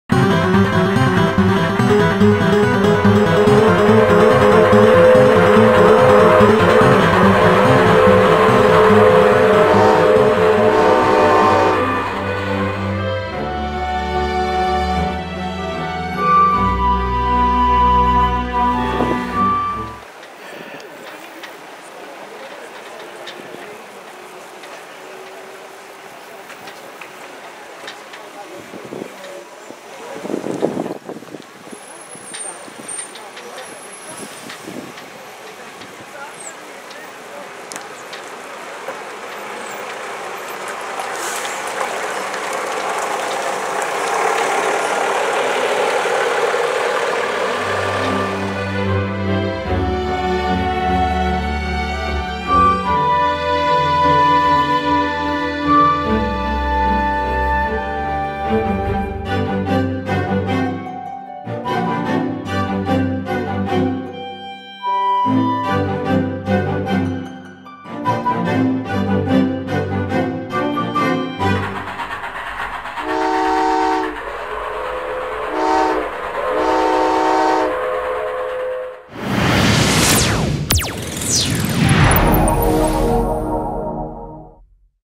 Brohltalbahn Bus
Hoch oben, am Bahnhof Engeln ist mir ein Bus untergekommen, ein ehemaliger Linienbus.